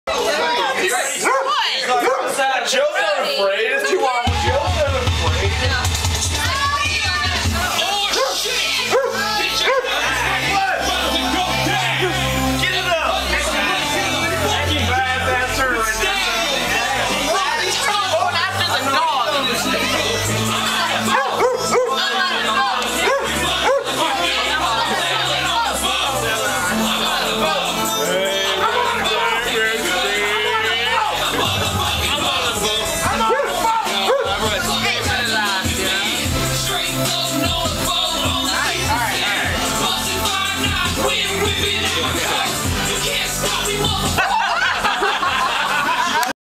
DOG IS FREAKED OUT BY JOE THE TURTLE!
DOG TRIES TO EAT TURTLE AT HOUSE PARTY